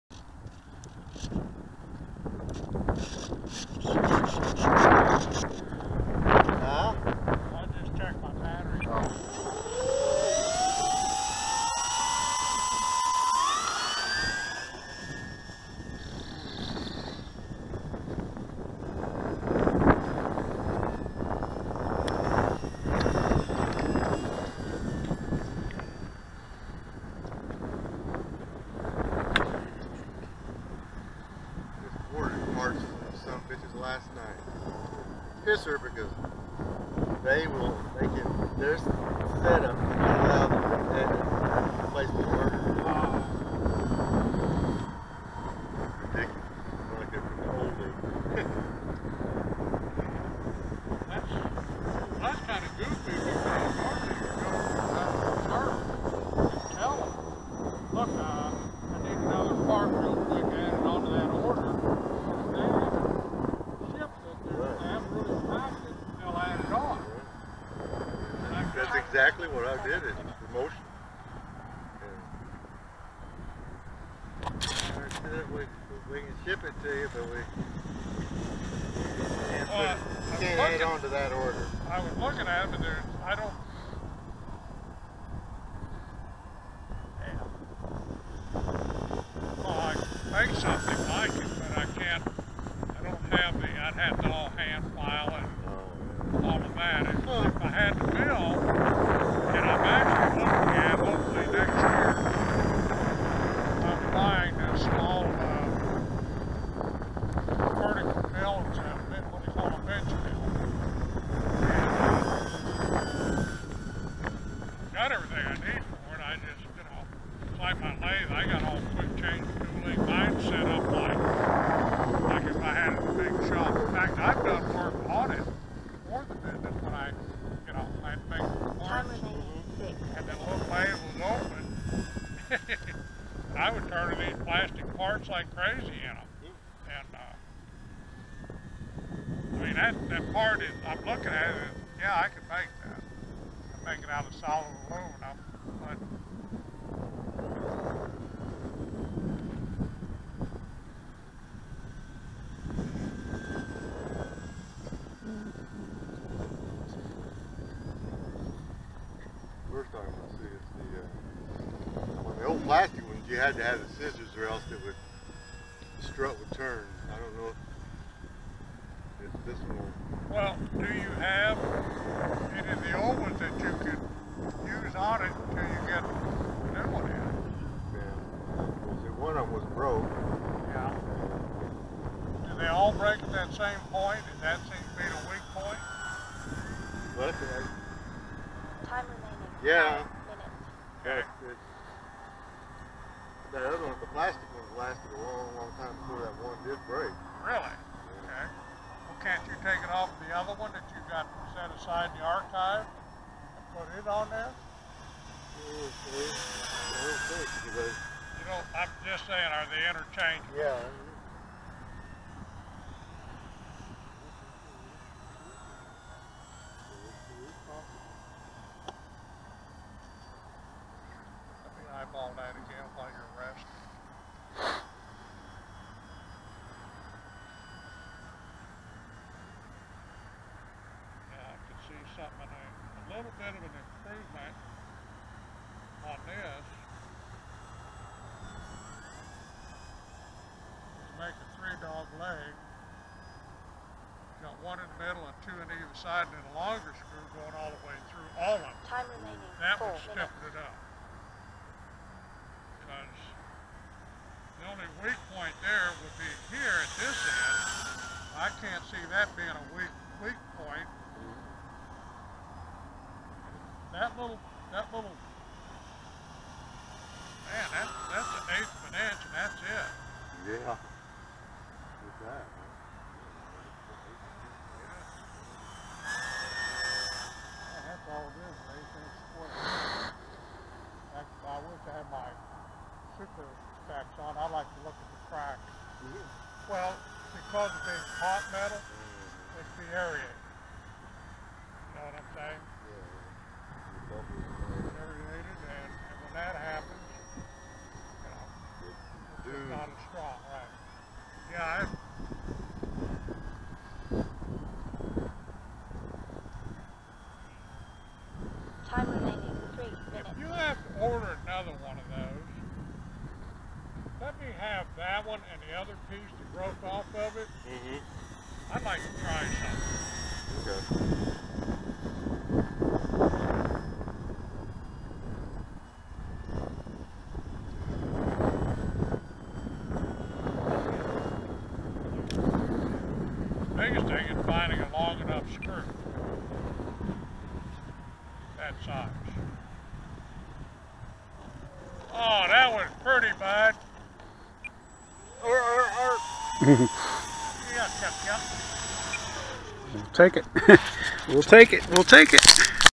E-Flite P-51 Mustang ASX Maiden 28Nov14
Maiden of the E-Flight Mustang ASX at Beautiful Aaron Bessant Park Panama City Beach, FL. Great flying plane, set up as specified by the manuel which is perfect. Plenty of power but looks best flying scale.